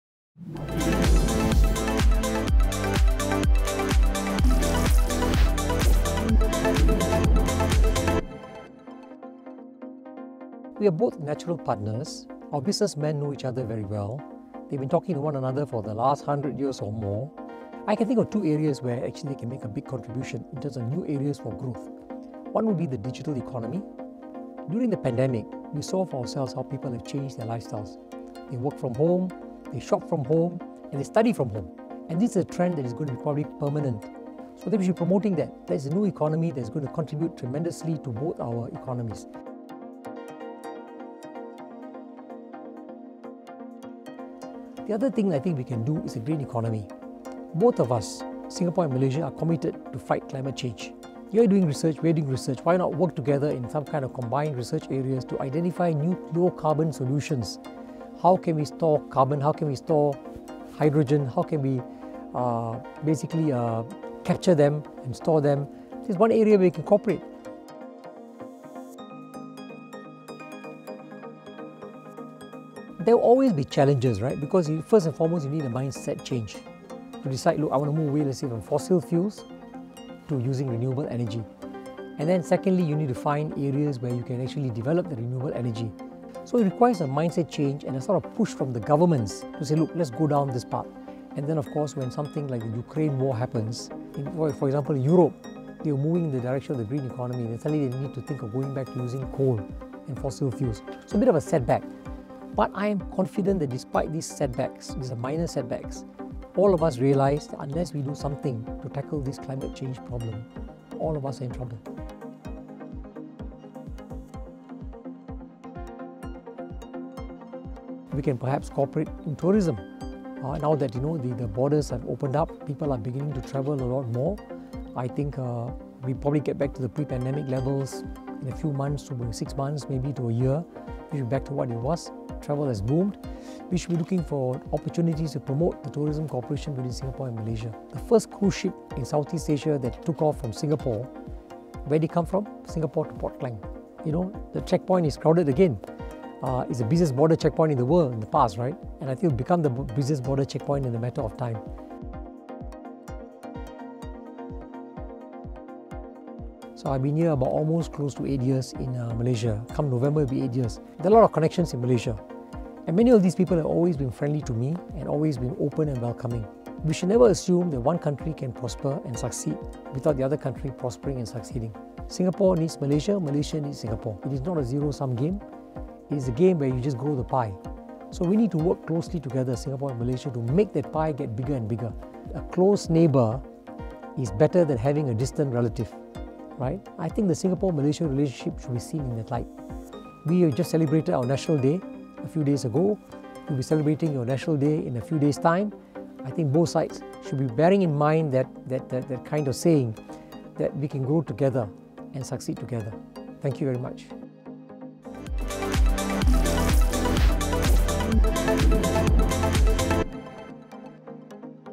发现东盟｜新加坡驻马最高专员 01 马新关系“远亲不如近邻”
发现东盟｜第四期  新马好兄弟好伙伴！

我们邀请到新加坡驻马来西亚最高专员梅侬进行专访，分享新马两国的兄弟情，以及对两国如何合作走向东盟，迎向世界的看法。